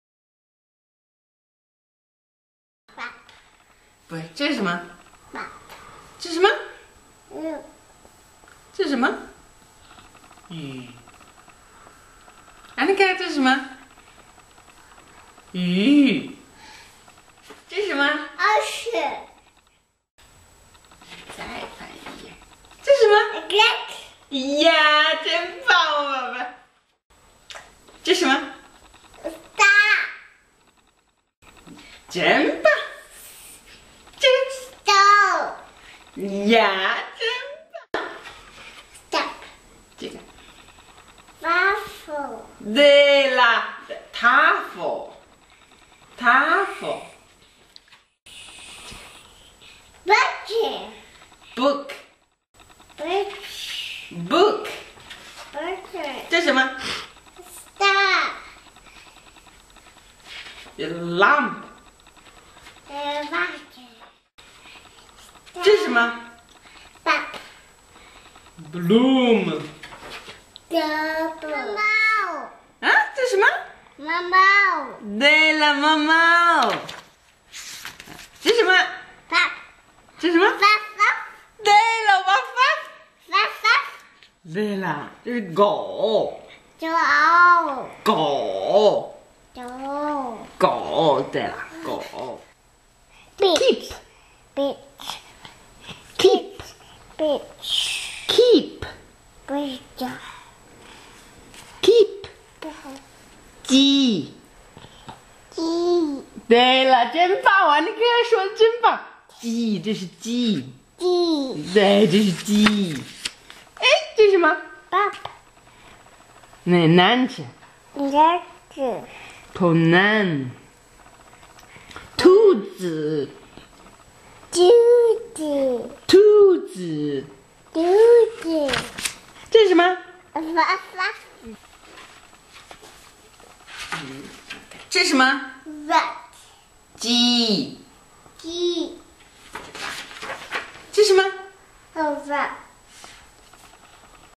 An Reading.wmv
An, 21 months old, learning new words